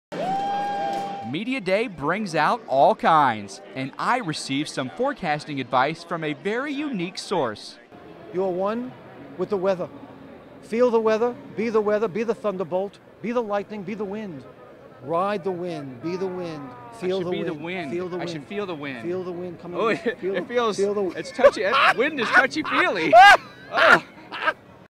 Strange Forecasters at the Super Bowl
The Weather Channel meteorologist Mike Bettes ran into some very unique personalities at the press day for the Super Bowl.